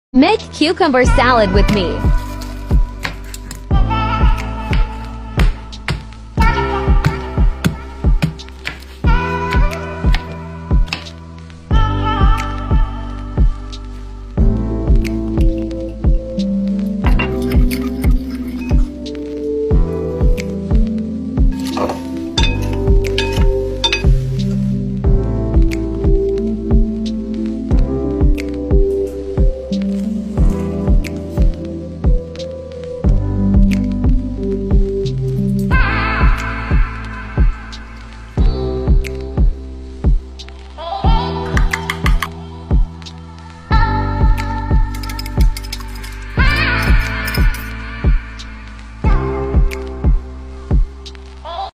Making Cucumber🥒Salads🥗 with Elena Yordan
A refreshingly healthy side dish for your hearty meal!🥒

Here is the recipe:

-2 pounds English cucumbers (about 2 large)
-4 teaspoons white sugar
Kosher salt
-4 teaspoons
-unseasoned rice vinegar
-1 medium garlic clove, peeled and smashed
-2 tablespoons grapeseed or other neutral oil
-½ teaspoon
-red pepper flakes
-1½ tablespoons soy sauce
-1 tablespoon toasted sesame oil
-1 tablespoon finely grated fresh ginger

✅ Follow me for more cookingwithessentialoils tips and tricks.